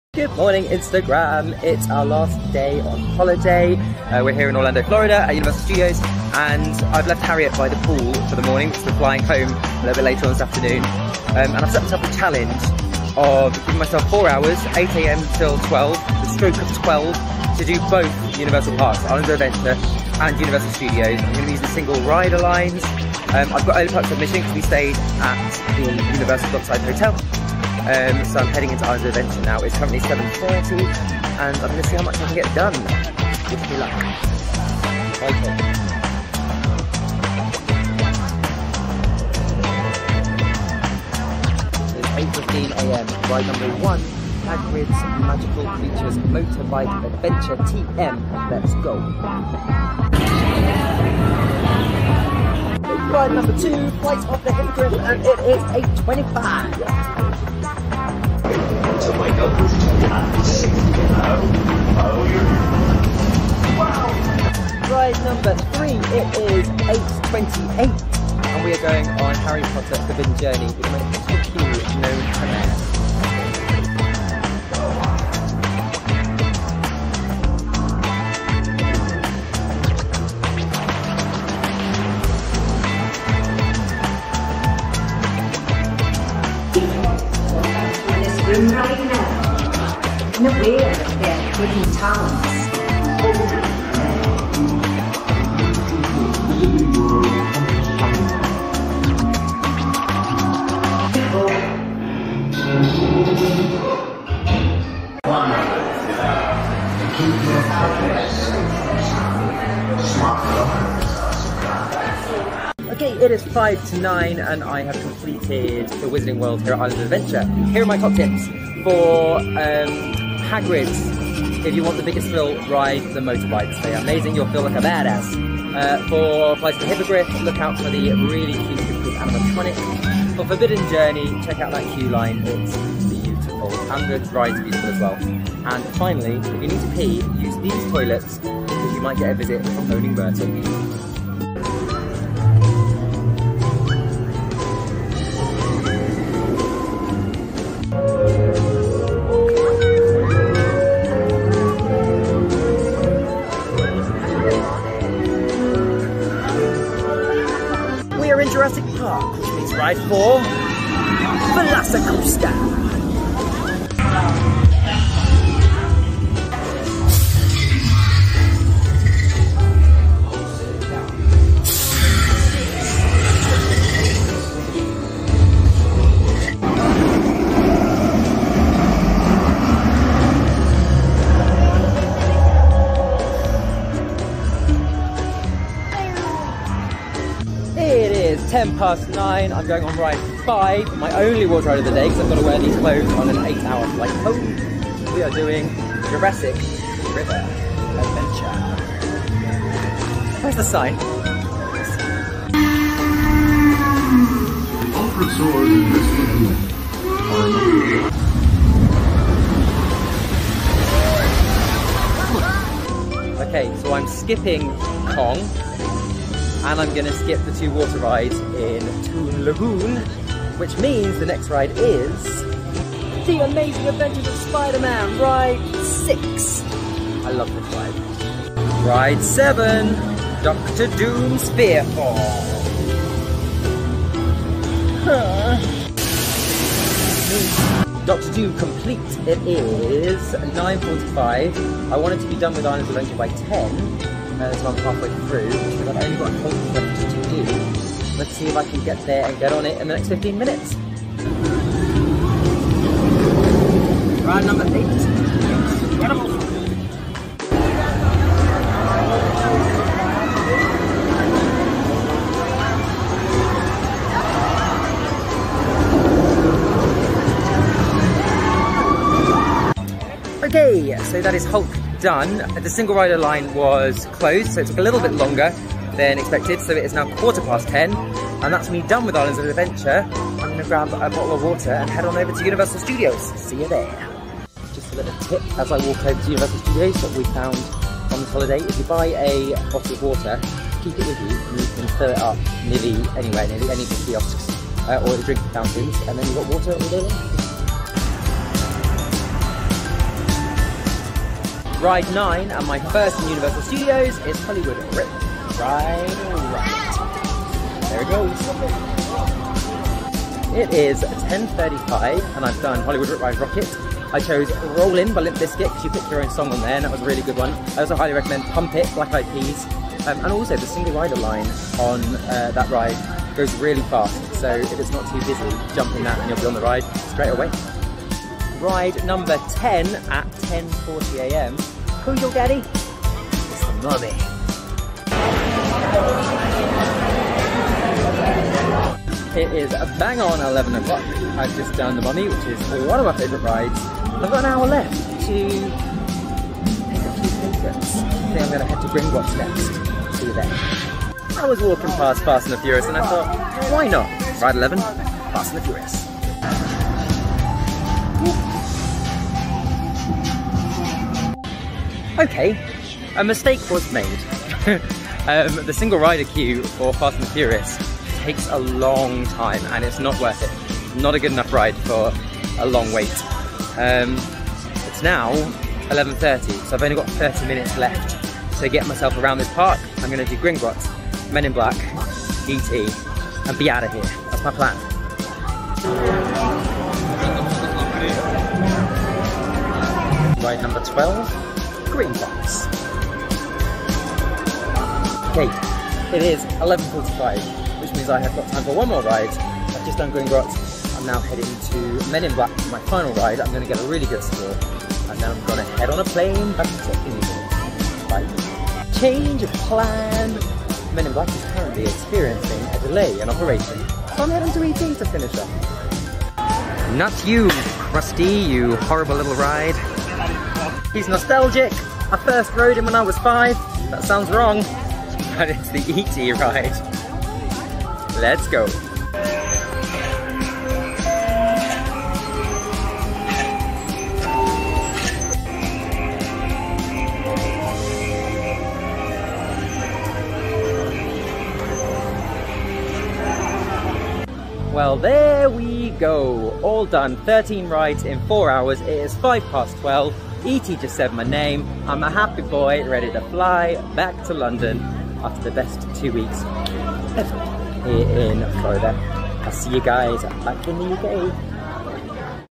UNIVERSAL PARKS DASH - BOTH PARKS & 13 RIDES IN JUST 4 HOURS!
I set myself the challenge of riding all of my favourite rides in both Universal Parks in just 4 hours!
How far did I get? Watch to find out, then copy my route!